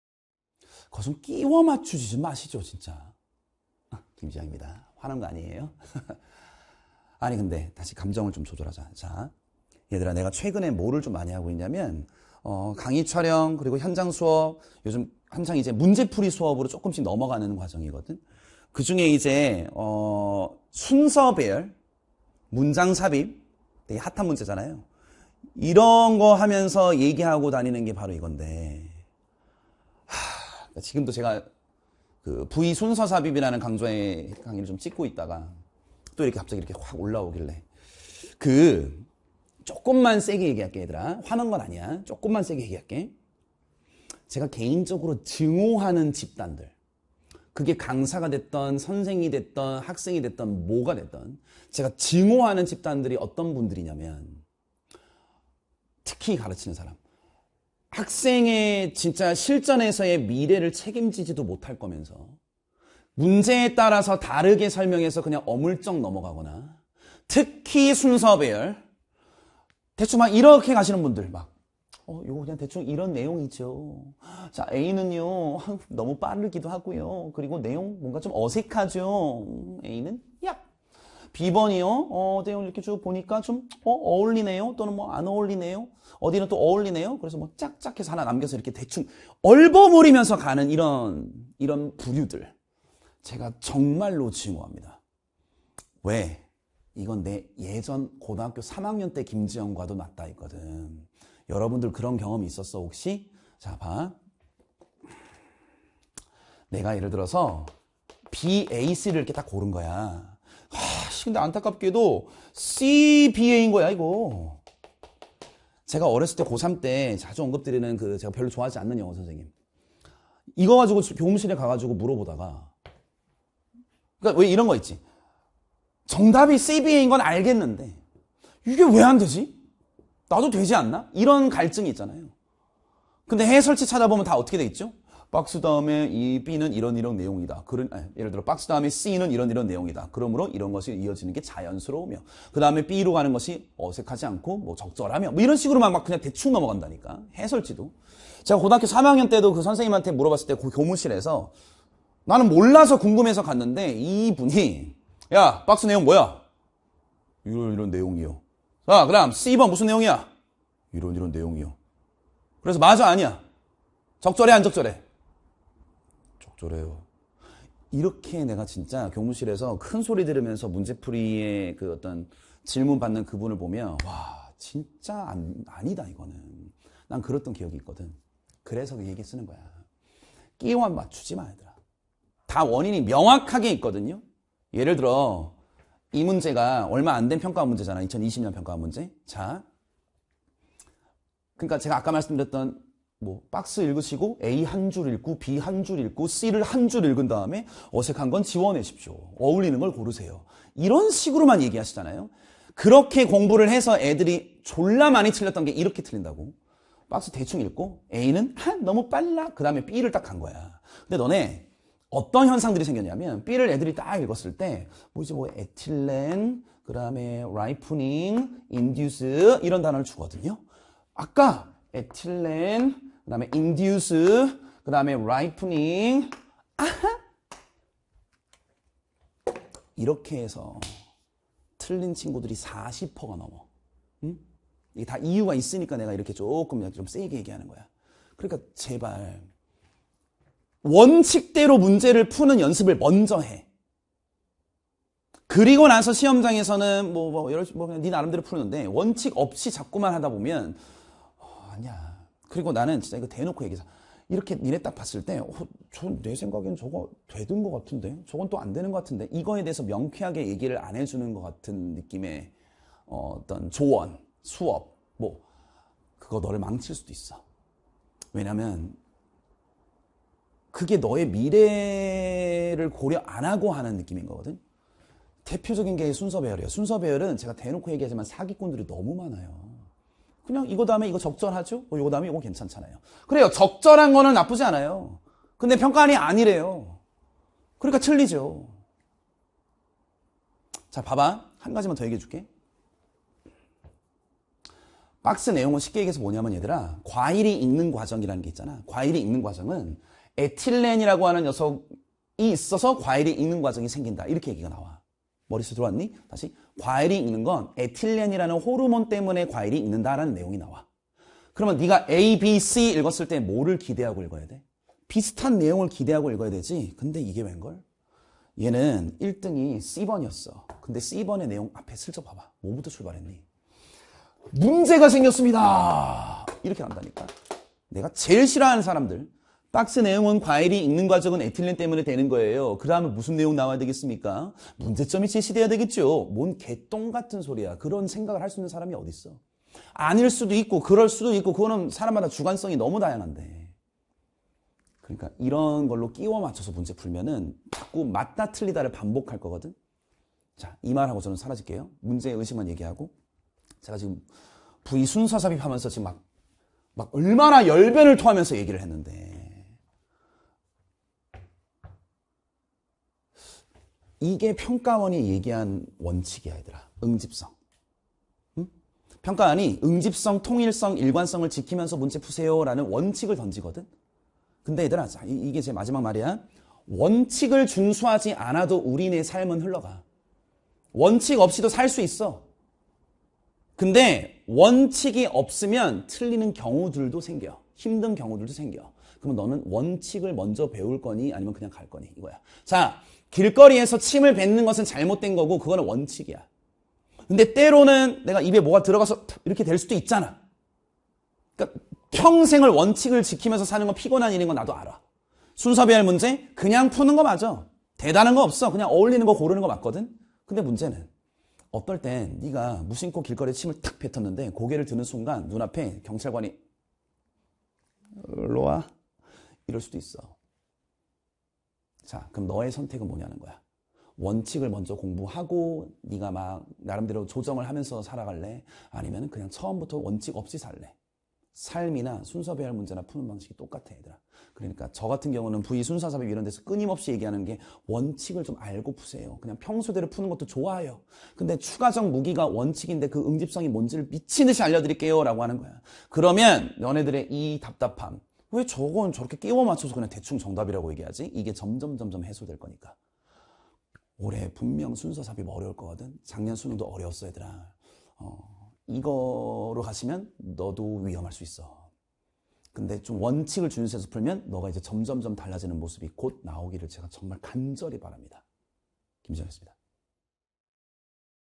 [메가스터디] 영어 김지영쌤 - 끼워 맞추지 좀 마!
순서배열 유형,
원칙대로 문제 푸는 연습부터
먼저 합시다!

원칙을 좀 알고 푸세요.
즉 ‘응집성’이 무엇인지를
알아야 합니다.

'왜 저건 저렇게 대충 끼워 맞춰서 정답이 되지?'
이러한 답답함이 해소될 겁니다.

자세한 내용은 영상을 통해 확인하기

- 영어 김지영 선생님을 더 알고 싶다면?